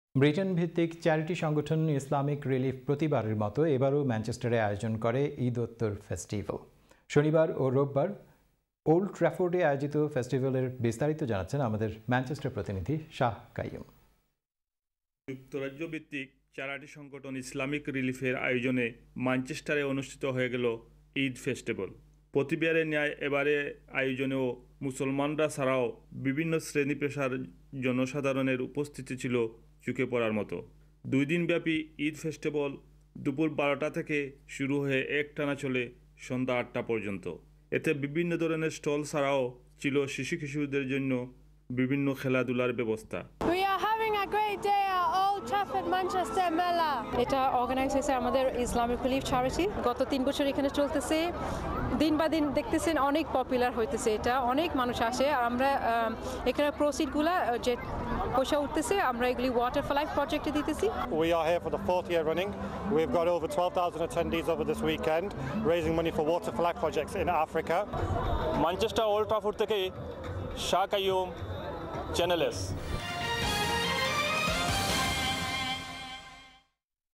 Islamic Relief Eid Festival at Old Trafford
যুক্তরাজ্যভিত্তিক চ্যারিটি সংগঠন ইসলামিক রিলিফ প্রতিবারের মতো এবারও ম্যানচ্যাস্টারে আয়োজন করে ঈদোত্তর ফ্যাস্টিভ্যাল। শনিবার ও রোববার ওল্ডট্রাফোর্ডে আয়োজিত ফ্যস্টিভ্যালের বিস্তারিত জানাচ্ছেন আমাদের ম্যানচ্যাস্টার প্রতিনিধি শাহ কাইয়ূম ।